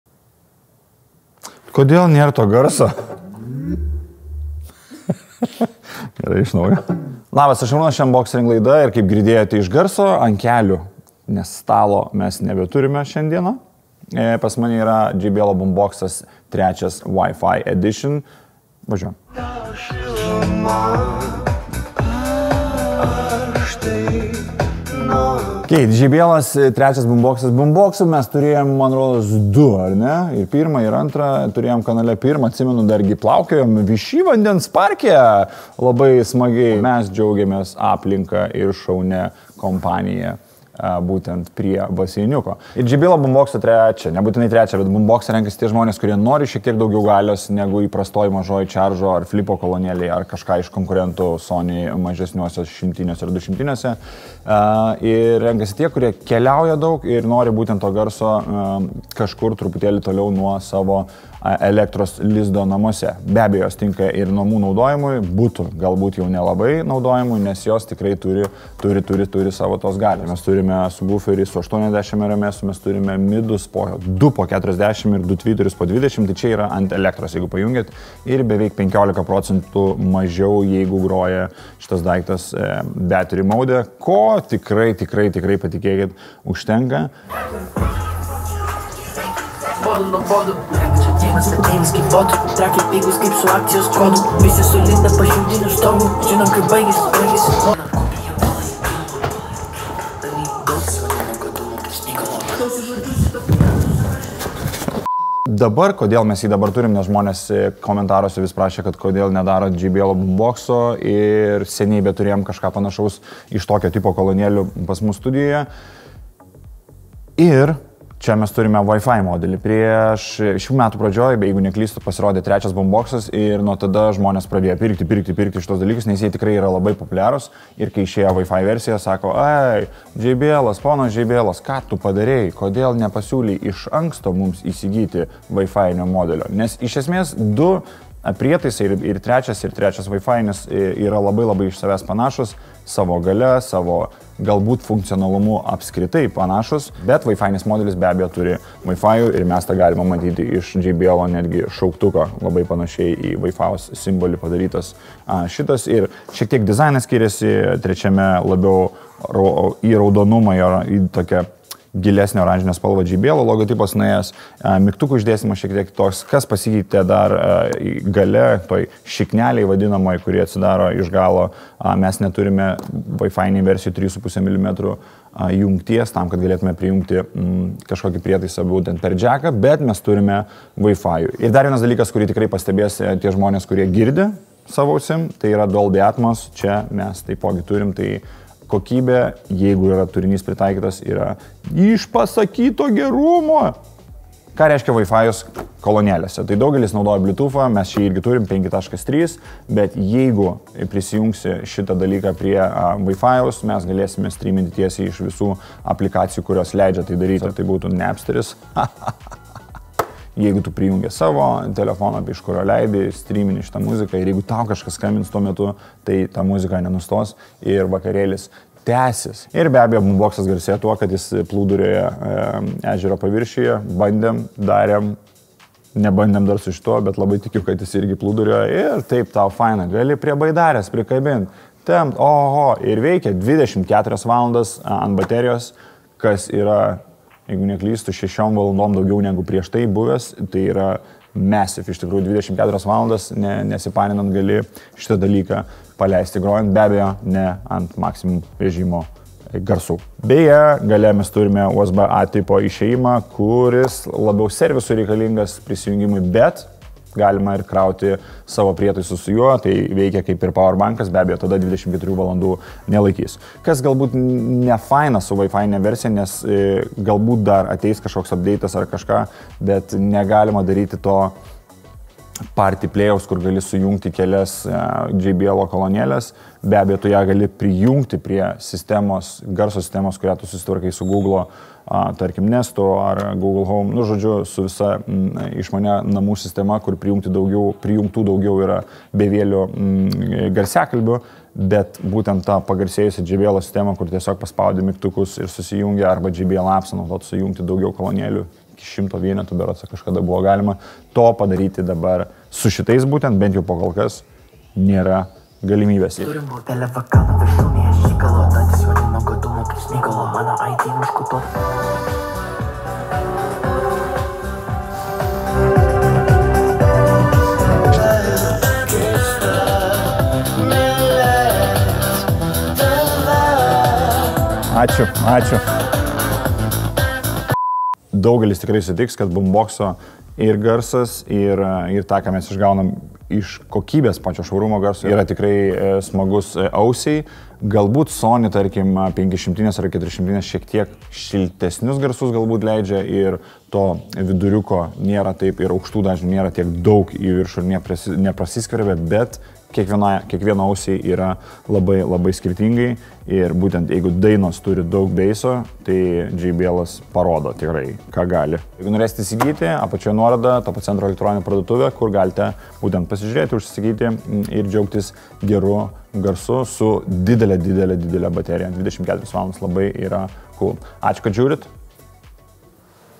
KOLONĖLĖ JBL BOOMBOX 3 + Wi-Fi | ŠOKIAMS LIETUJE & VAKARĖLIUI PO VANDENIU || UNBOX RING
🥊 Nešiojamą kolonėlę JBL Boombox 3, Bluetooth + Wi-Fi galite įsigyti galite įsigyti TOPO CENTRE
Šarūno rankose – trečios serijos JBL Boombox + Wi-Fi modelis:

Kolonėlės įtaka keisti atmosferą, geriausiai atsispindi paties laidos šeimininko nuotaikoje bekuriant šį epizodą.

Vos tik išmėginsite patys – viską suprasite, o jeigu tai jau padarėte, pasidalinkite patirtimi!

„Laisvės TV X" – lietuviškas YouTube kanalas